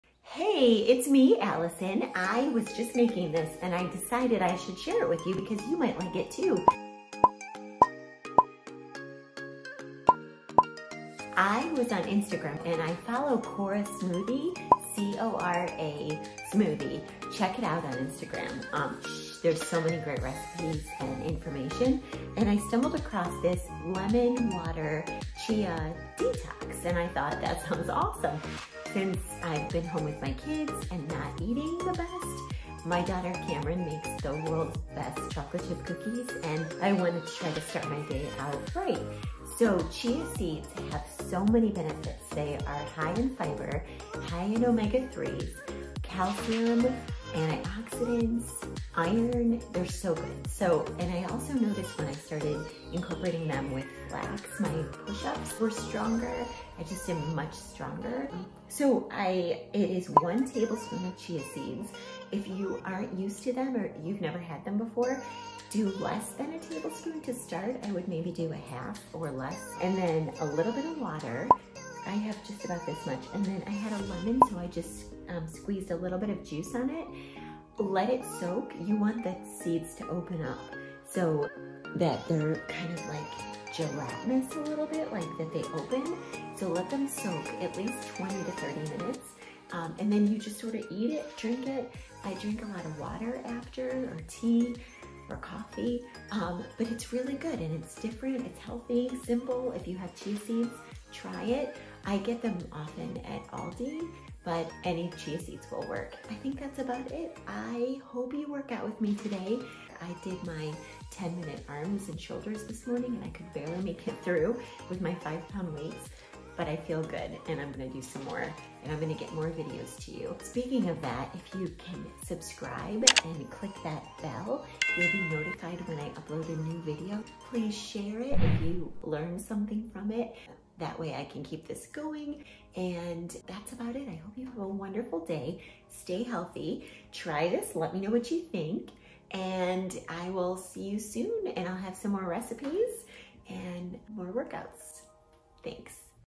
CHIA SEED LEMON WATER
Hi! I want to share my new discovery with you!! Chia seeds are a superfood and so good with lemon water! Check it out!